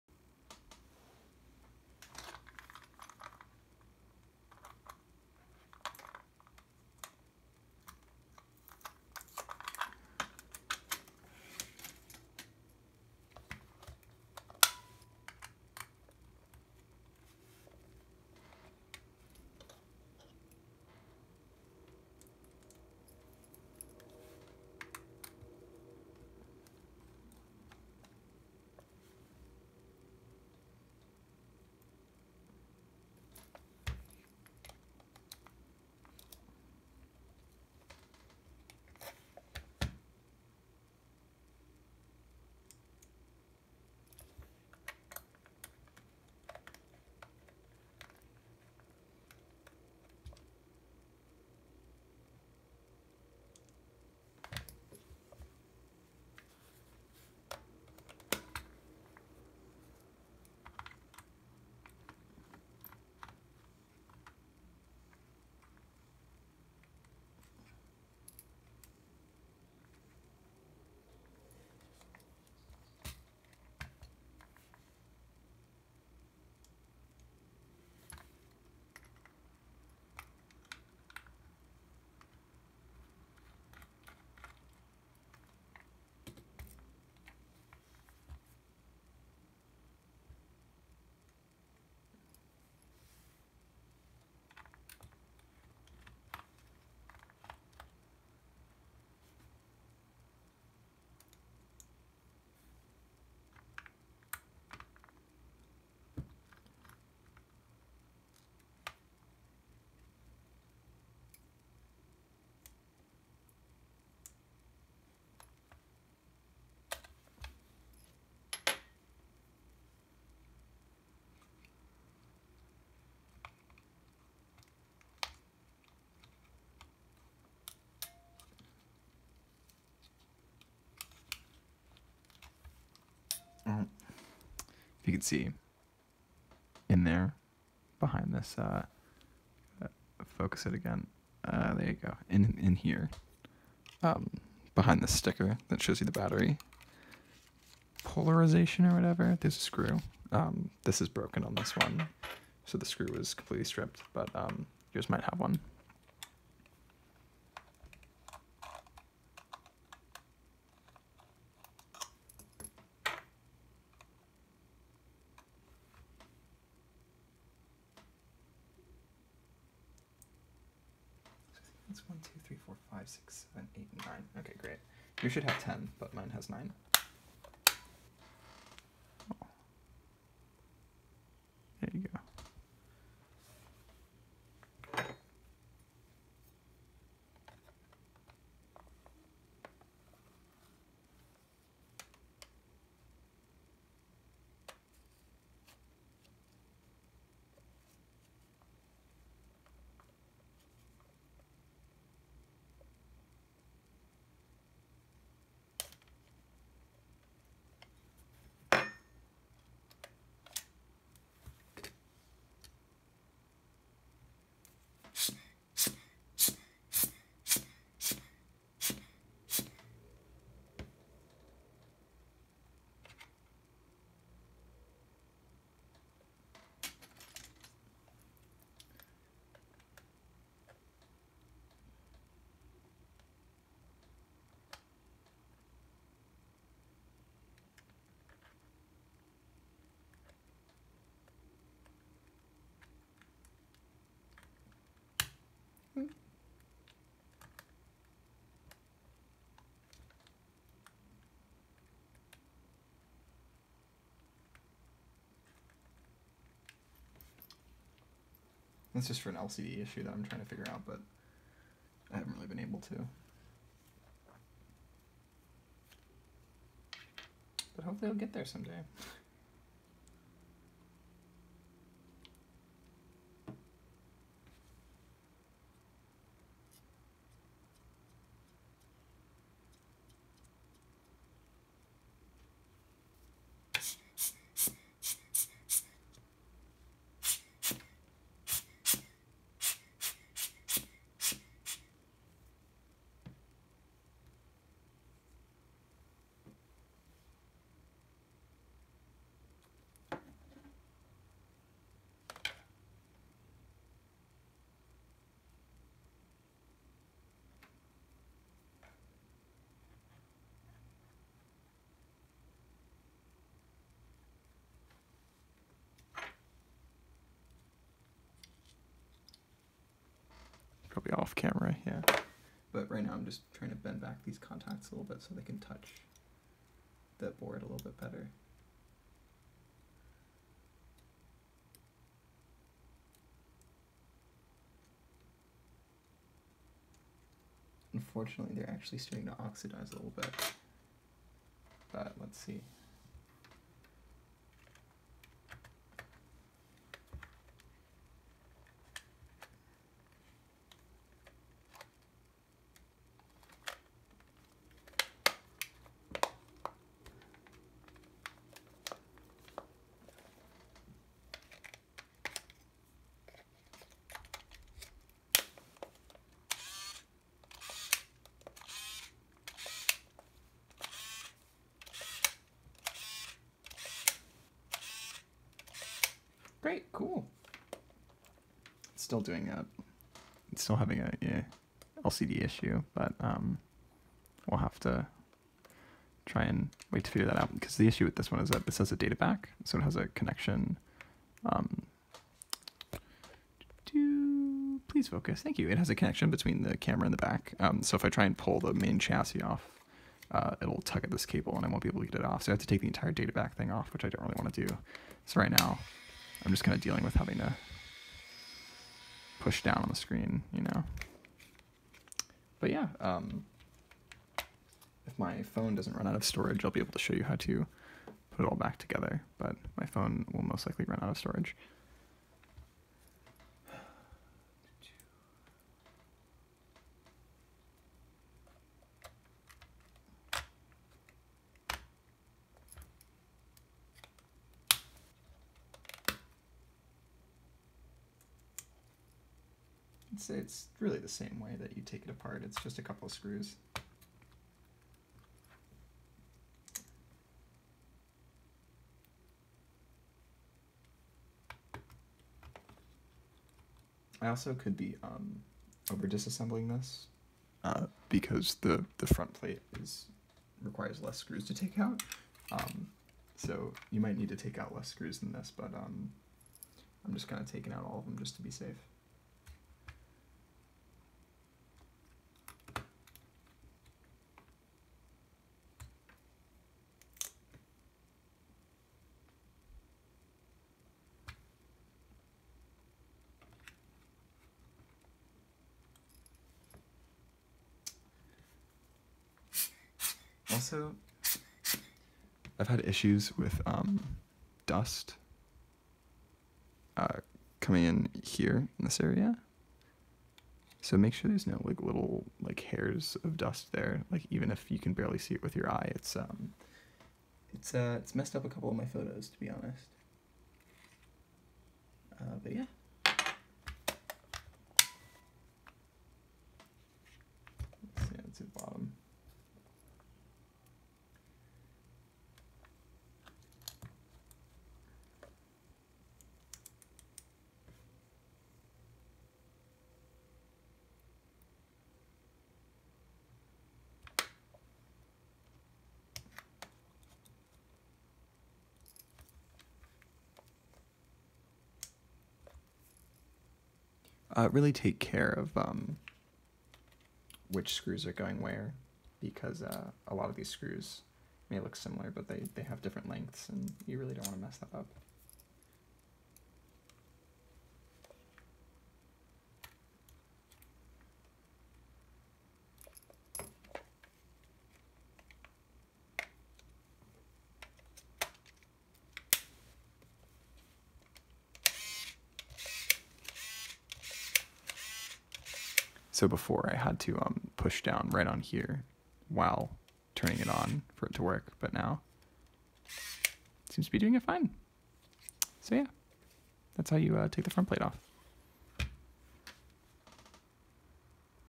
Yashica T4 On/Off Repair (Minor)
Here's a little video going over the removal of the front plate of a Yashica T4 in order to access the metal brushes that contact the board in order to turn the camera off. The lack of solid contact was obvious, and the lens would sometimes stay protracted after turning the switch off. When removing the front plate, all that was necessary was to clean the brush contacts and board with isopropyl alcohol and and small bending out of the brush contacts so that they had a better connection to the board.